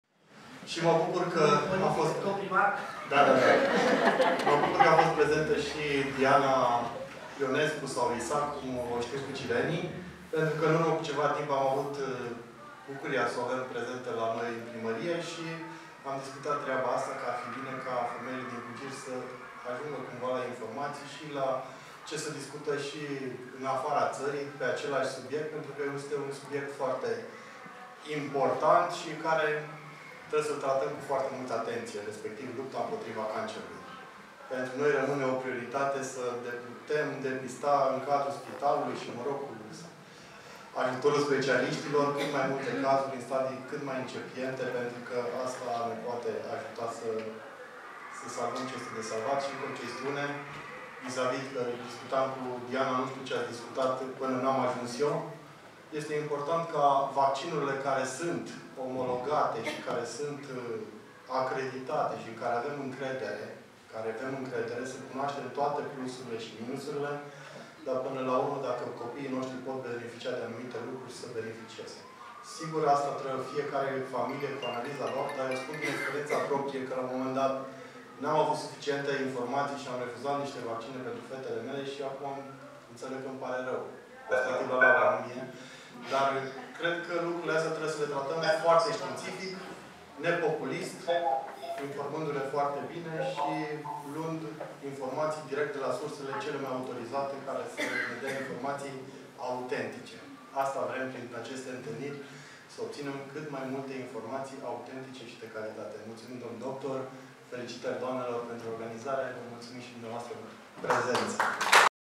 Dl primar al Cugirului, Adrian Teban
Intervenție în cadrul evenimentului "Superfemei", organizat de doamna Mariana Pereanu, cu sprijinul doamnelor din PNL Cugir